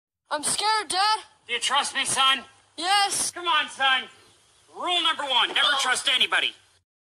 SMTIVA animatic: Dagda in a nutshell
Made another video today, wasn't planning to but this fits too well with Dagda. Another title for this would be Nanashi learns why none of the other Irish Gods like Dagda

(Also rip anatomy)